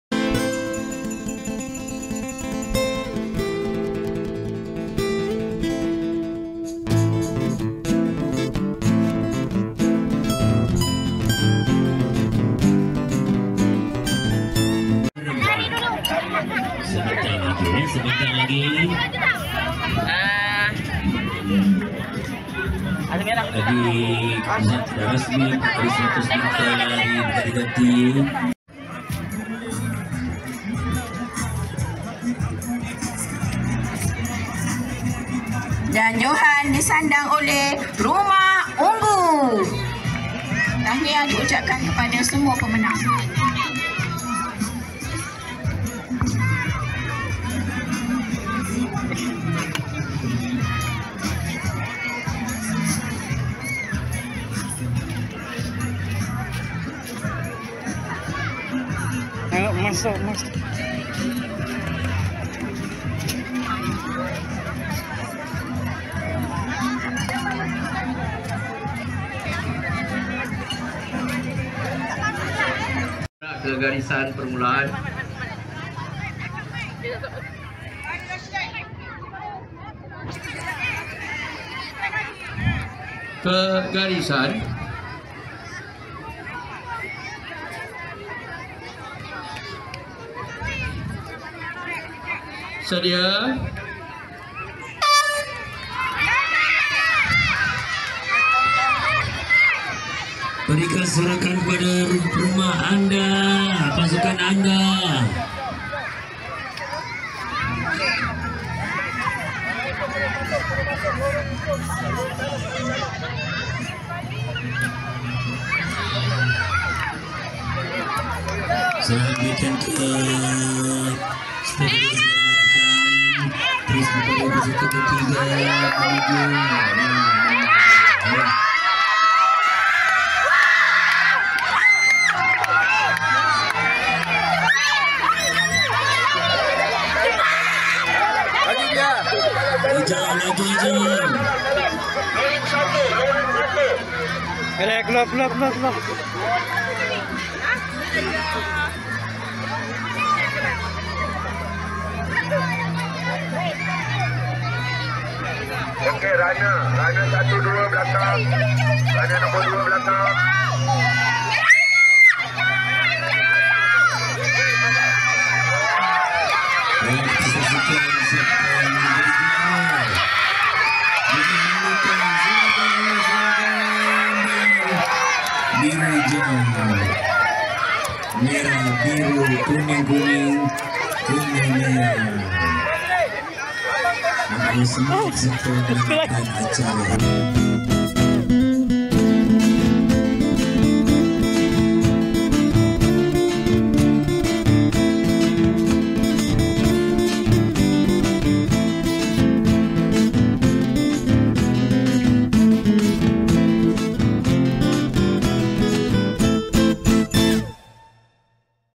SUKAN SKBC 2023 - 4x100m Perempuan (T4)
Keputusan :
Emas -Team  Biru (Pahlawan)
Perak  - Team Hijau (Perkasa)
Gangsa - Team Merah (Pendekar)

Tahniah kepada semua pemenang.